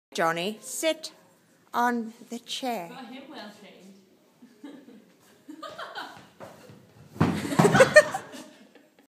Three legged chair
Experimenting with distorting and deconstructing chairs in order to change people's psychological state.